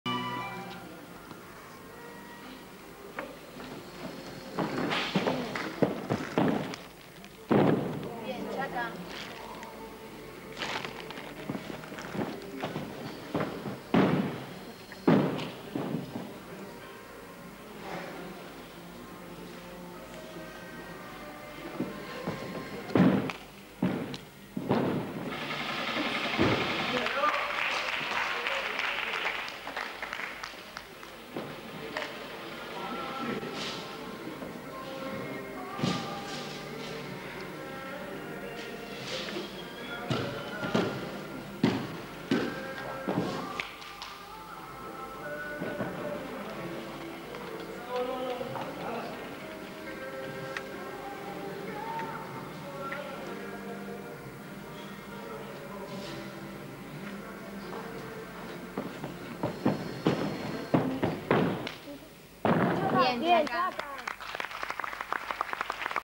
1997 Argentina National Championships - Andrés Orsini (GEBA) FX (Argentina TV)
Argentina's TyC Sports coverage of Argentina's 1997 National Championships.

Copyright is not mine, captured from a TyC Sports broadcast from 2000.